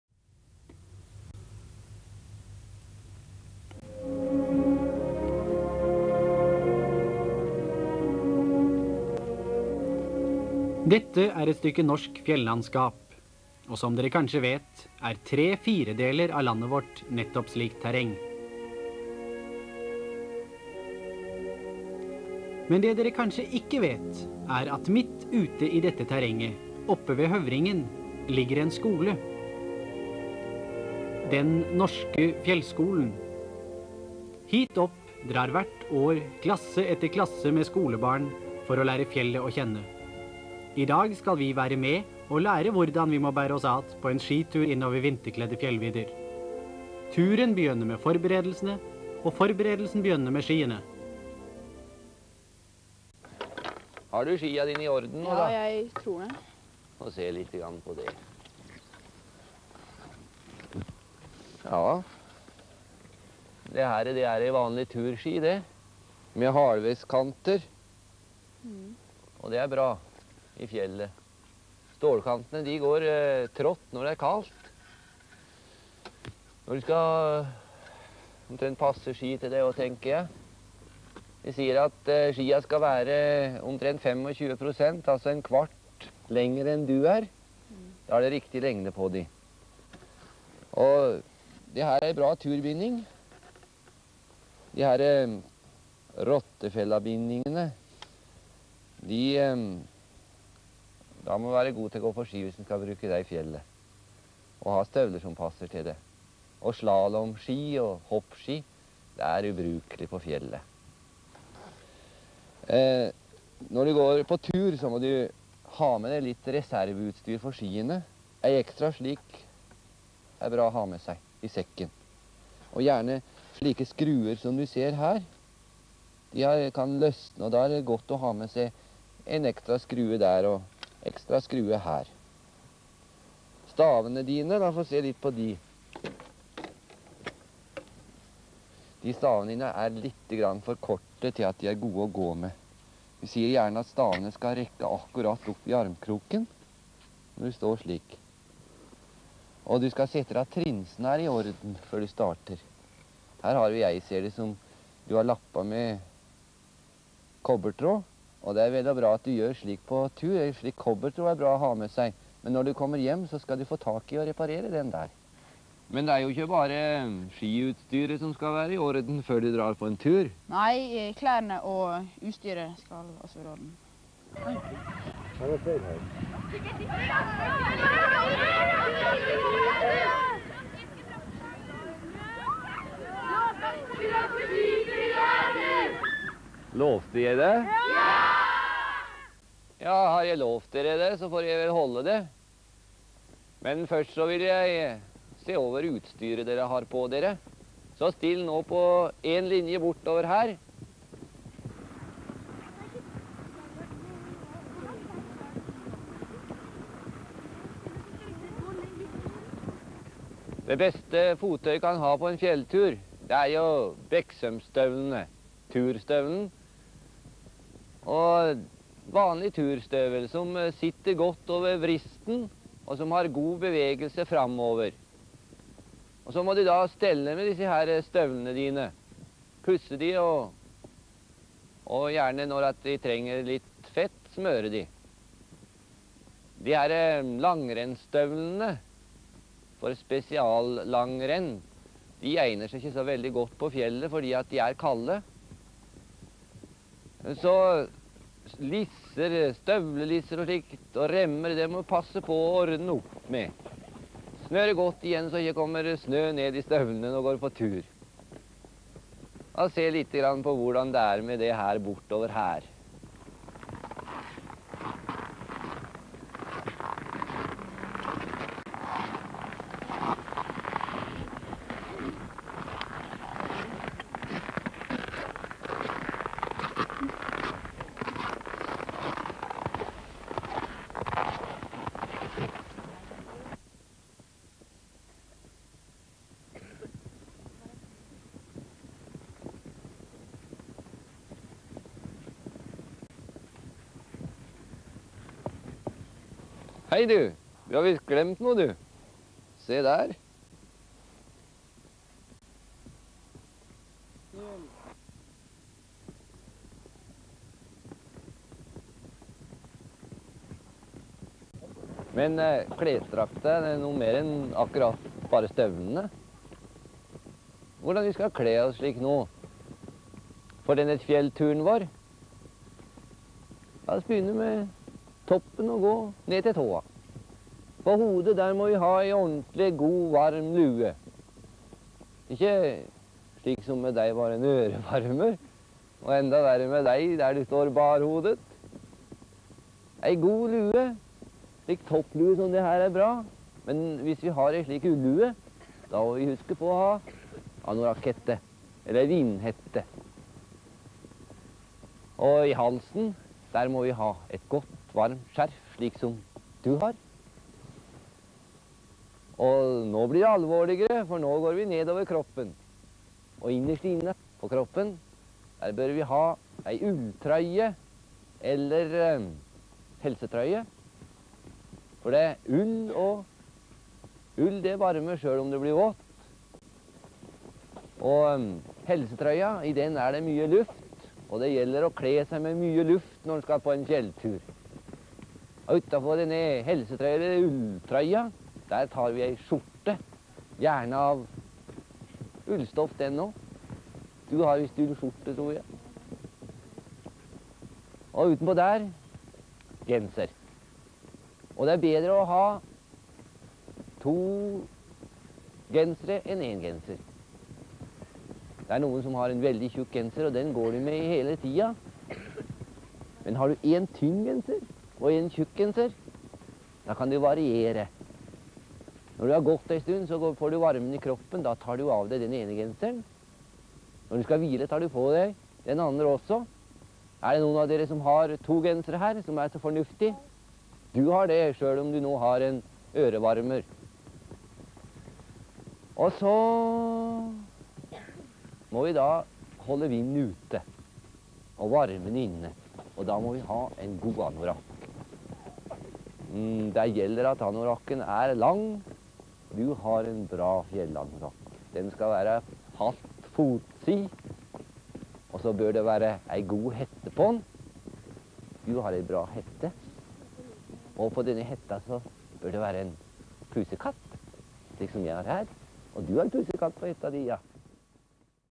1965 Den Norske Fjellskolen - Forberedelser til tur 1
Onkel Terje forteller om hva man skal ha på seg, hva skal være med i sekken, om skiutstyr og hvordan smøre ski.